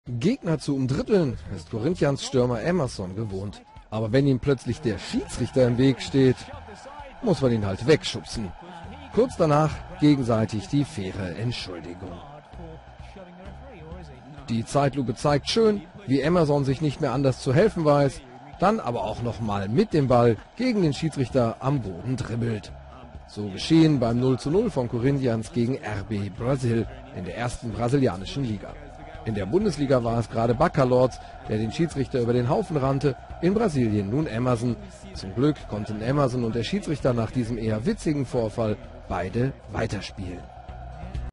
Spieler schubst Schiedsrichter aus dem Weg
Am Wochenende standen die Schiedsrichter offensichtlich besonders oft im Weg: In Frankfurt rannte Bakalorz Schiri Winkmann um - in Brasilien schubste Emerson
den Schiedsrichter aus dem Weg.

Da hat wohl einer Bundesliga geschaut: Ähnlich wie Paderborns Marvin Bakalorz gegen Schiedsrichter Guido Winkmann hat auch Corinthians Emerson den Schiedsrichter zu Fall gebracht.